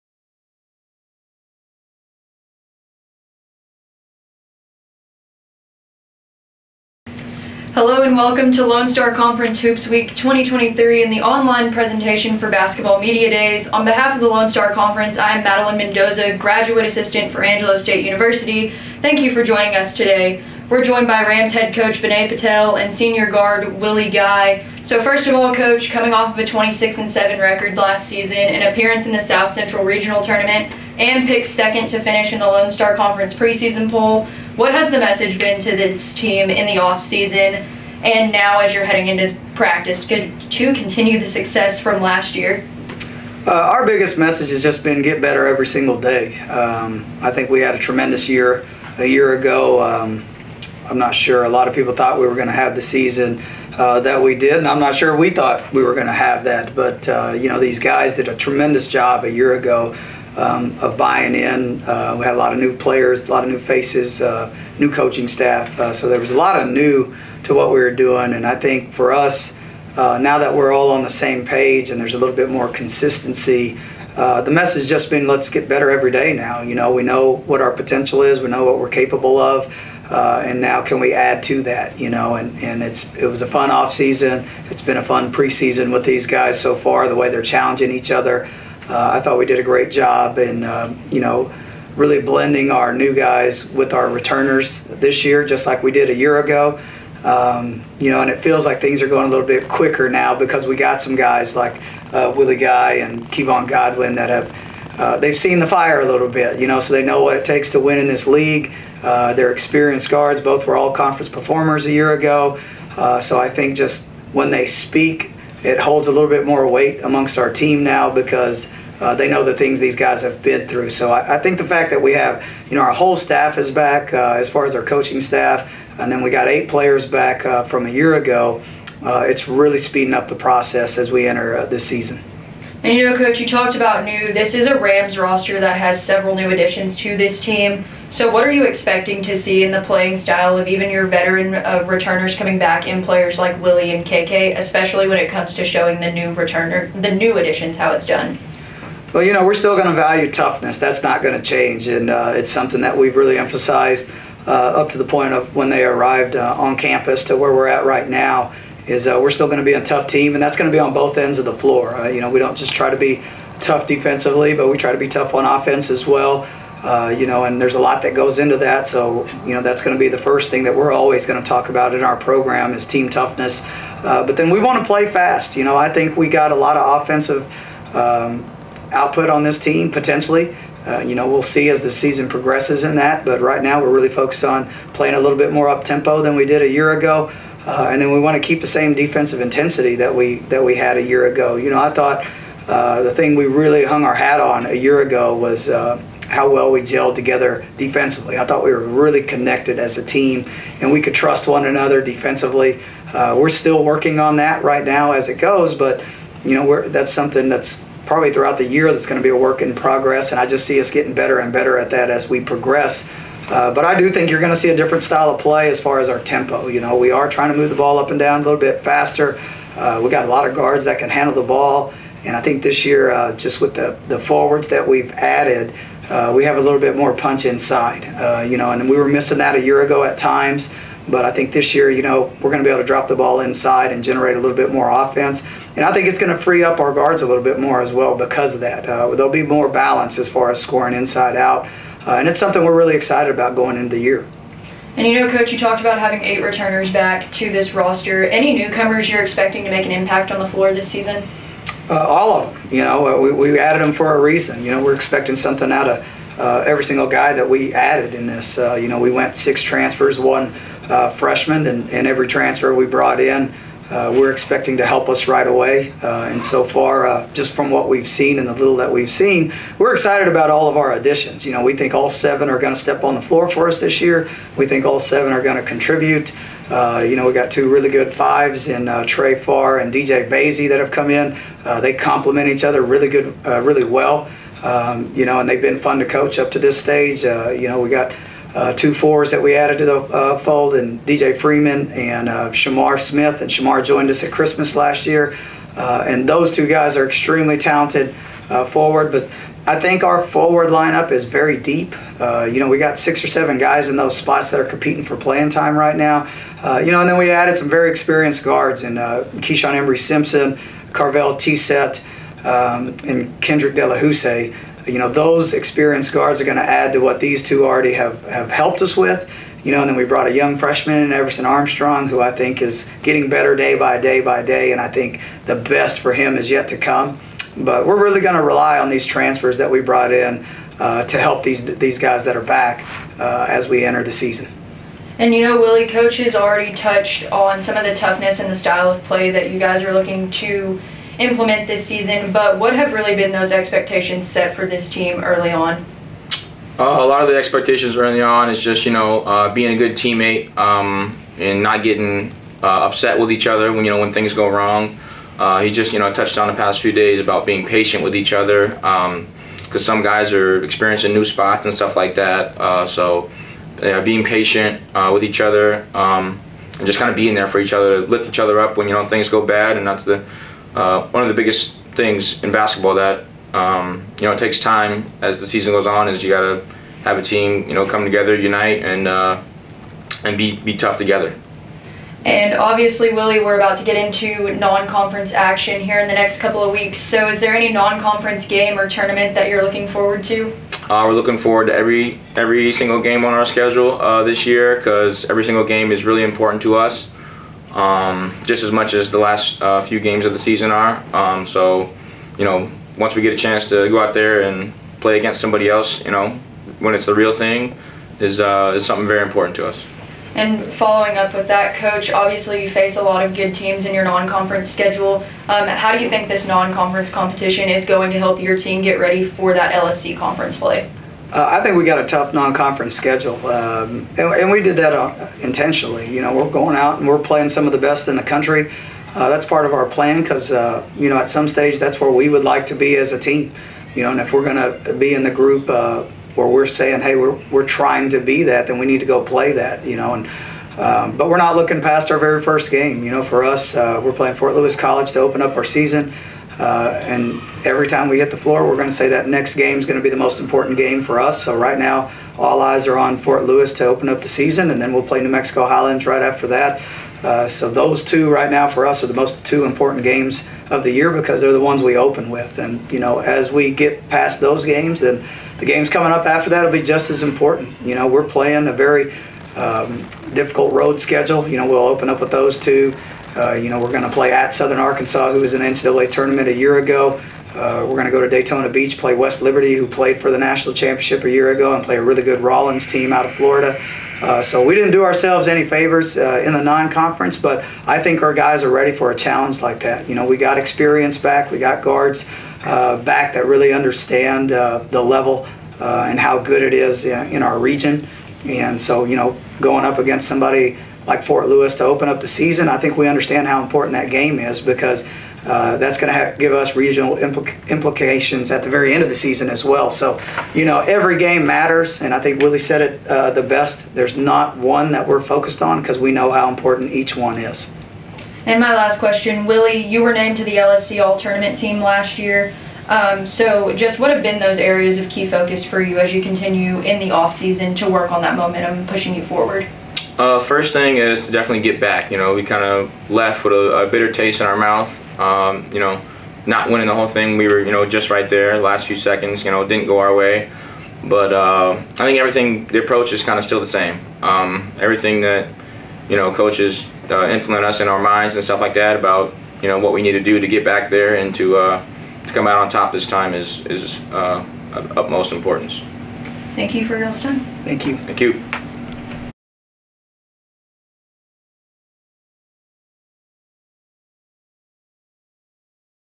Angelo State Men's Basketball (2023 LSC Basketball Online Media Days)
Head Coach Vinay Patel and senior guard Willie Guy.